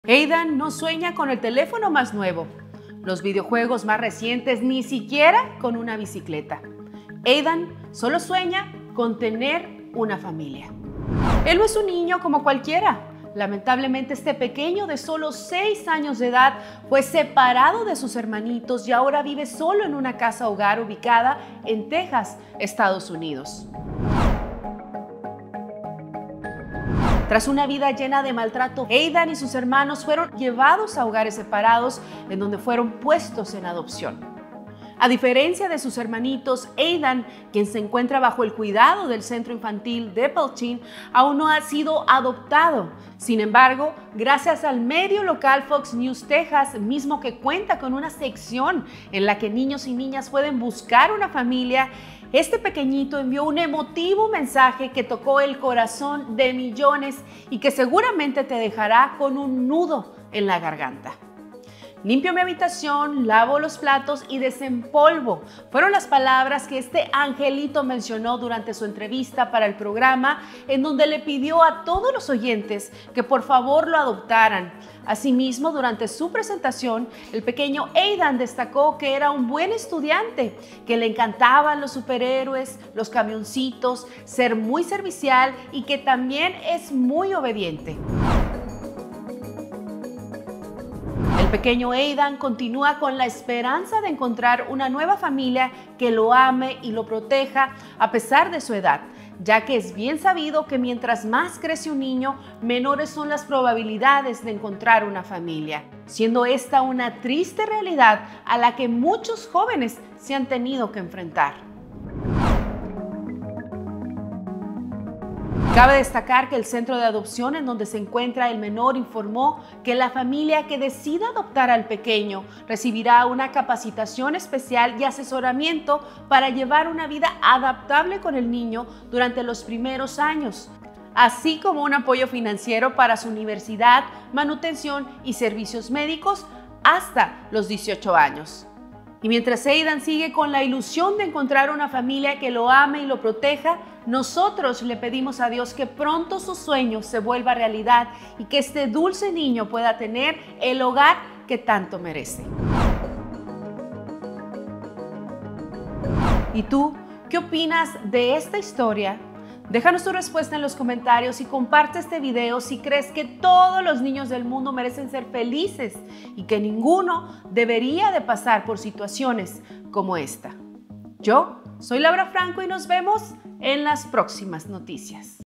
Niño ruega ser adoptado. “Limpio mi cuarto y lavo los platos
Uno de los momentos más difíciles que podría tener un niño es separado de su hermanos para ser enviado a una casa de hogar y buscar un hogar e convierte en una difícil reto ¿Qué opinas de esta historia?

BADABUN NETWORK ©